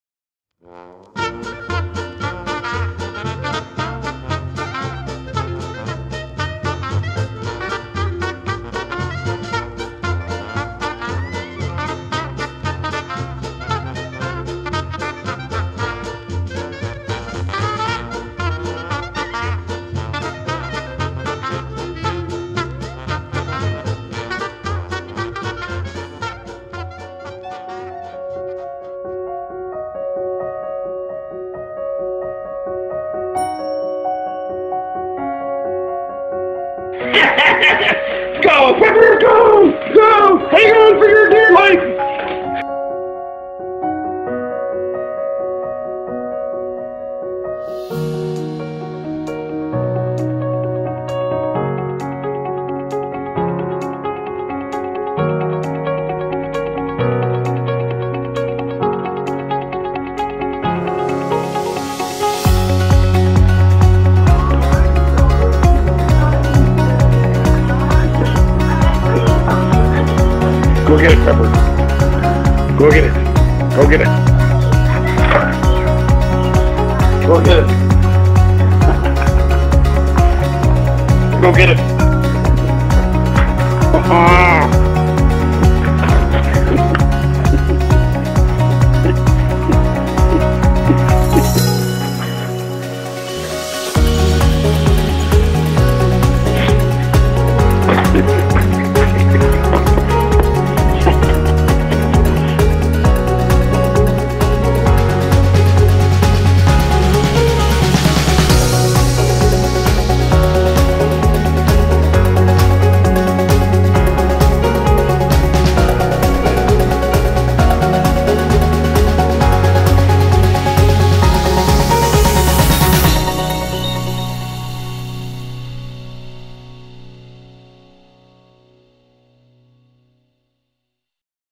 more pup pepper fun
Larry reminisces about when Pepper (aka Beepis) was a pup.  Good stuff!